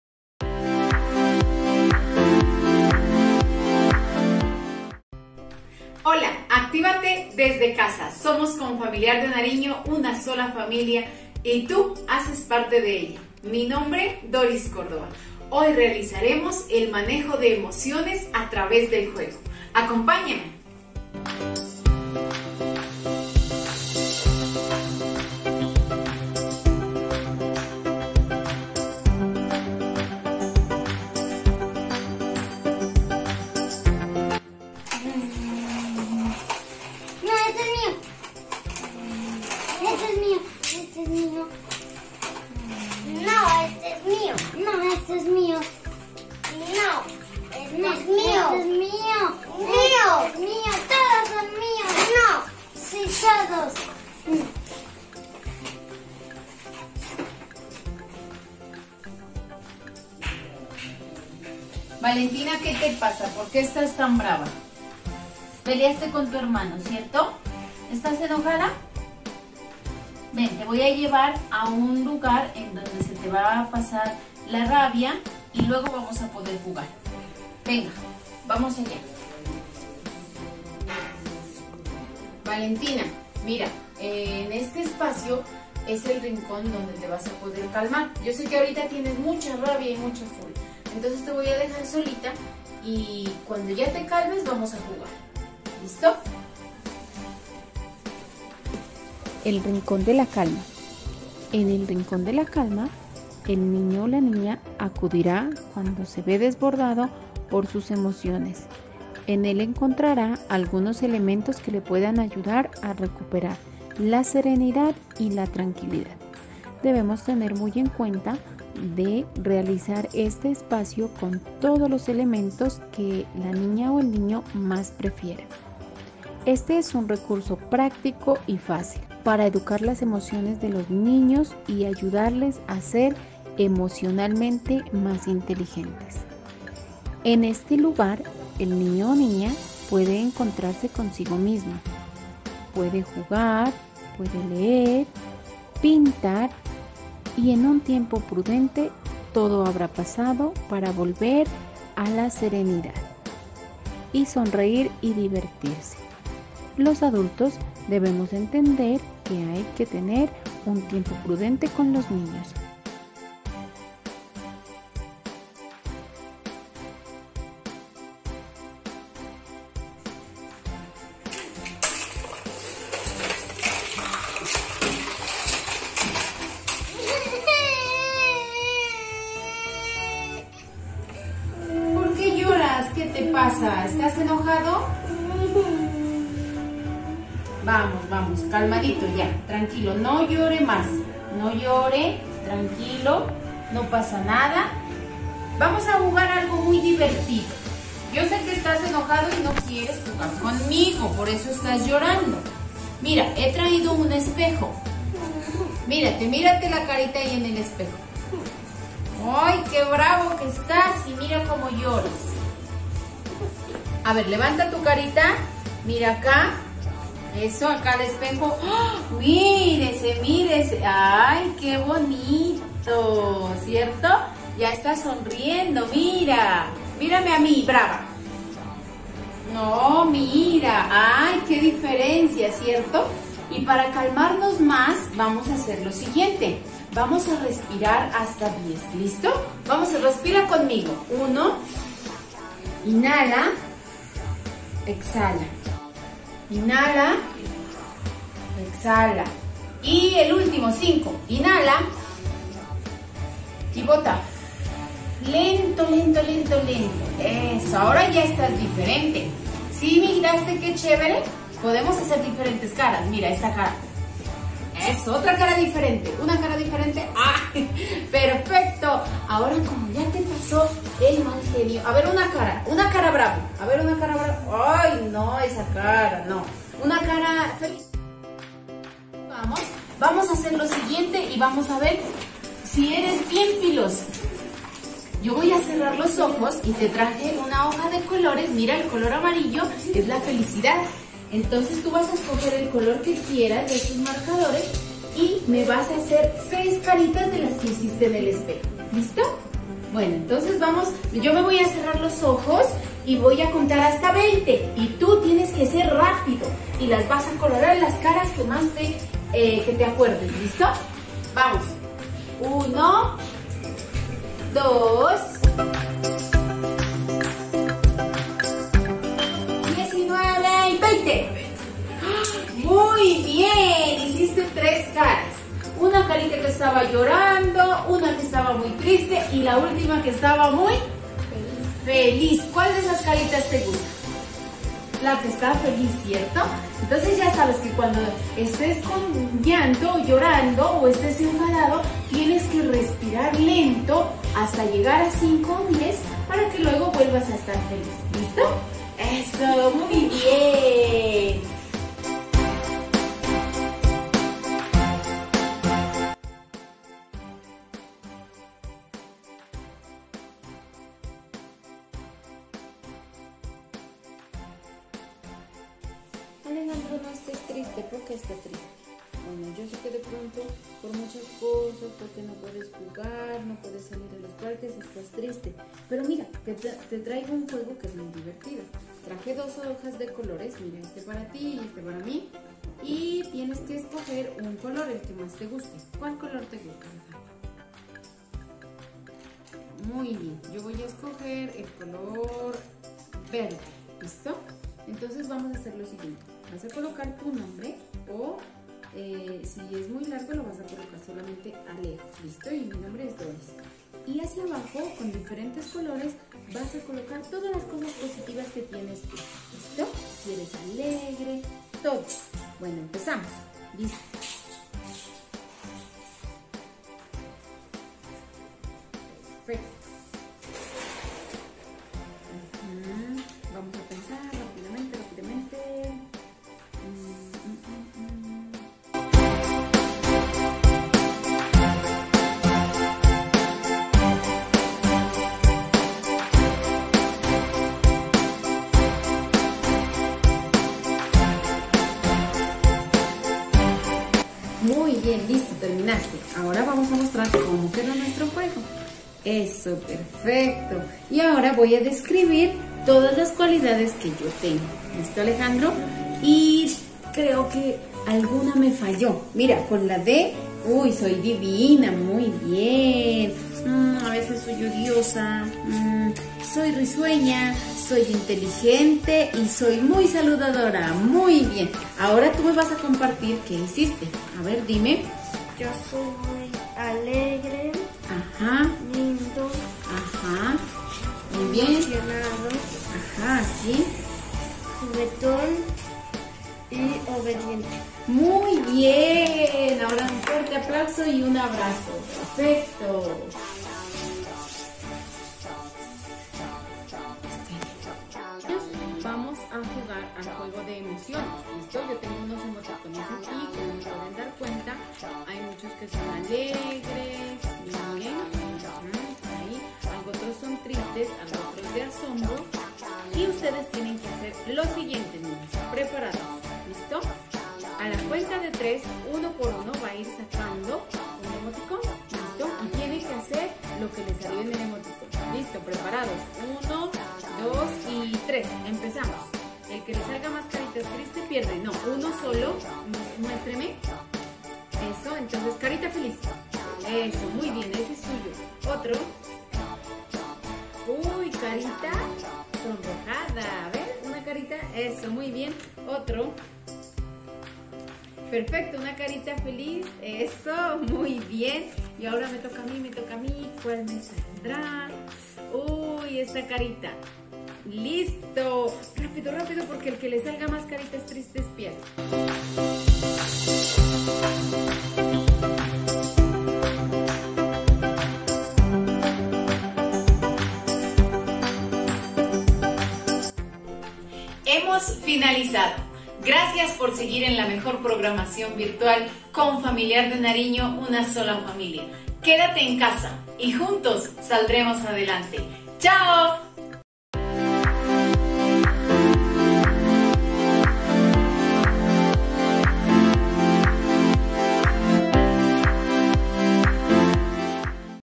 Estrategias lúdicas: manejo de emociones a través del juego, instructora Doris Córdoba
ÁREA INFANTIL Y RECREACIÓN FAMILIAR: Estrategias lúdicas: manejo de emociones a través del juego, instructora Doris Córdoba